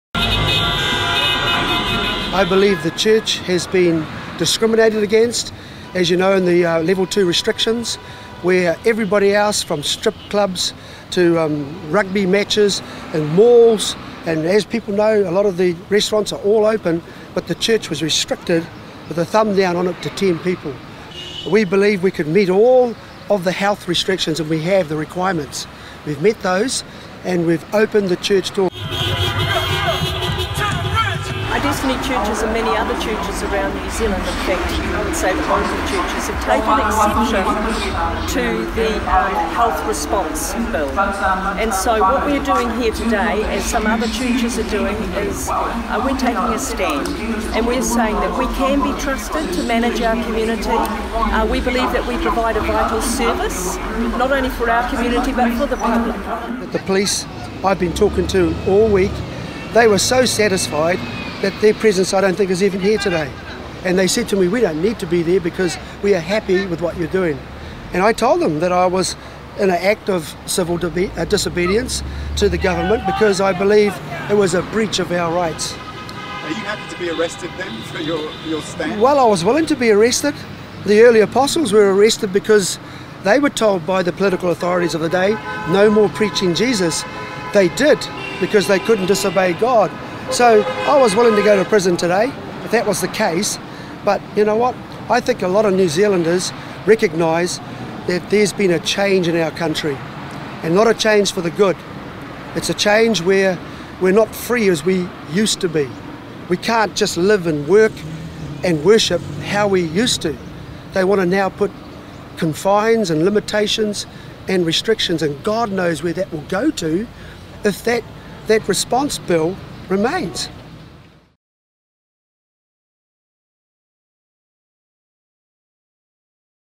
Destiny Church goes ahead with service despite cap on on religious gatherings | nzherald.co.nz
Tamaki billed Sunday’s service as taking a stand for the rights of everyone in New Zealand, including the right to religious freedom with a 10-person cap on religious gatherings in alert level 2, which the Government says is necessary to prevent the spread of Covid-19. Full story

Video / Dean Purcell